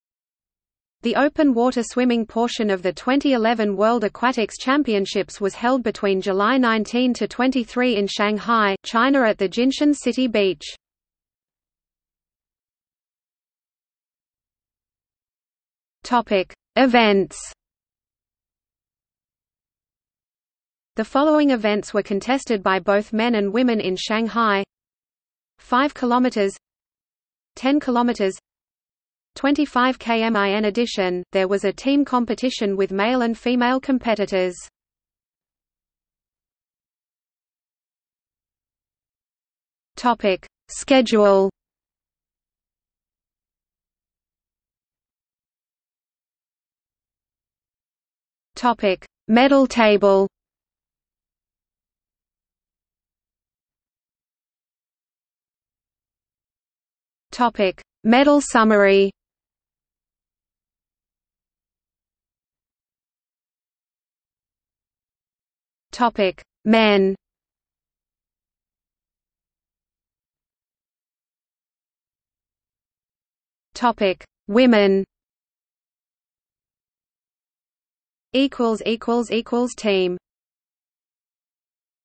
Open water swimming at the 2011 World Aquatics Championships | Wikipedia audio article
00:00:12 1 Events
00:00:36 2 Schedule
00:00:45 3 Medal table
00:00:53 4 Medal summary
00:01:02 4.1 Men
00:01:10 4.2 Women

- Socrates

SUMMARY
The open water swimming portion of the 2011 World Aquatics Championships was held between July 19–23 in Shanghai, China at the Jinshan City Beach.